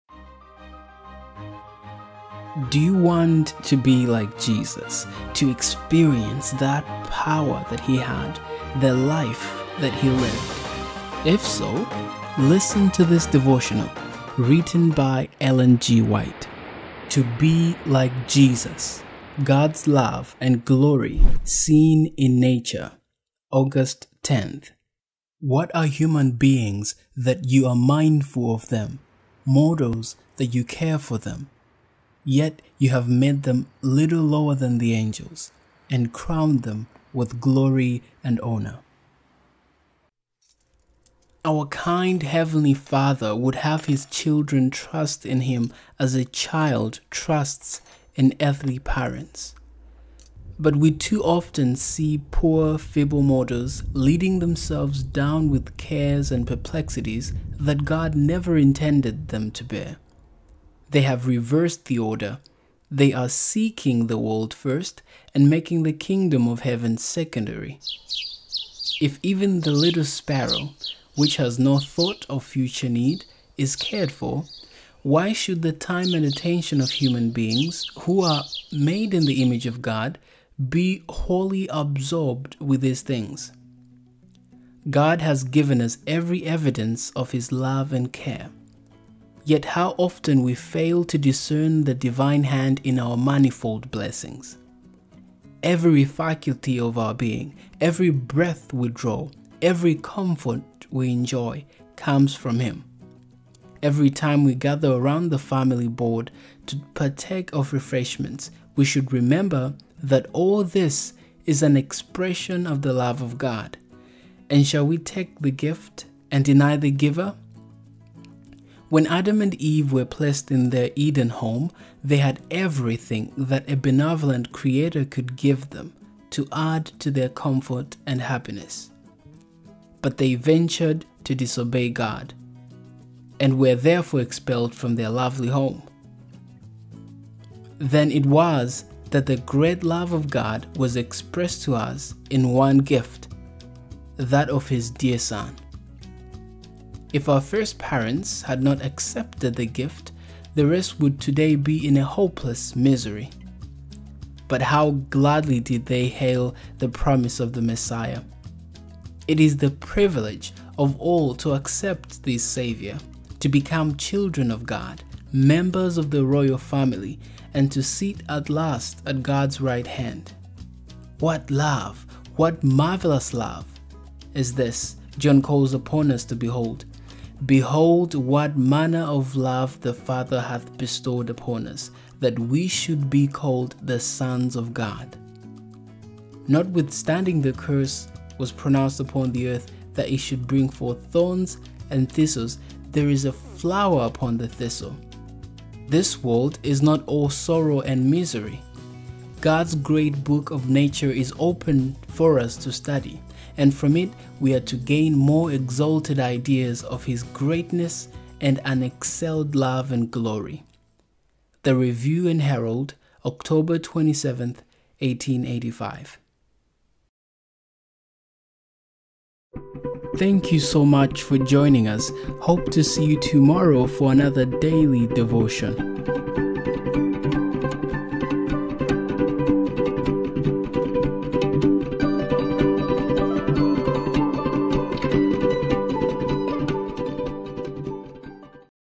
God's Love and Glory Seen in Nature, August 10
What are human beings that you are mindful of them, mortals that you care for them? Yet you have made them a little lower than angels [margin], and crowned them with glory and honor. Psalm 8:4, 5, NRSV. Our kind heavenly Father would have His children trust in Him as a child trusts in earthly parents. But we too often see poor, feeble mortals loading themselves down with cares and perplexities that God never intended them to bear. They have reversed the order; they are seeking the world first, and making the kingdom of heaven secondary. If even the little sparrow, which has no thought of future need, is cared for, why should the time and attention of human beings, who are made in the image of God, be wholly absorbed with these things? God has given us every evidence of His love and care, yet how often we fail to discern the divine hand in our manifold blessings. Every faculty of our being, every breath we draw, every comfort we enjoy, comes from Him. Every time we gather around the family board to partake of refreshments, we should remember that all this is an expression of the love of God. And shall we take the gift, and deny the Giver? ... When Adam and Eve were placed in their Eden home, they had everything that a benevolent Creator could give them to add to their comfort and happiness. But they ventured to disobey God, and were therefore expelled from their lovely home. Then it was that the great love of God was expressed to us in one gift, that of His dear Son. If our first parents had not accepted the gift, the race would today be in hopeless misery. But how gladly did they hail the promise of the Messiah. It is the privilege of all to accept this Savior, to become children of God, members of the royal family, and to sit at last at God's right hand. What love, what marvelous love, is this! John calls upon us to behold it: “Behold, what manner of love the Father hath bestowed upon us, that we should be called the sons of God.” Notwithstanding the curse was pronounced upon the earth that it should bring forth thorns and thistles, there is a flower upon the thistle. This world is not all sorrow and misery. God's great book of nature is open for us to study, and from it we are to gain more exalted ideas of His greatness and unexcelled love and glory.—The Review and Herald, October 27, 1885.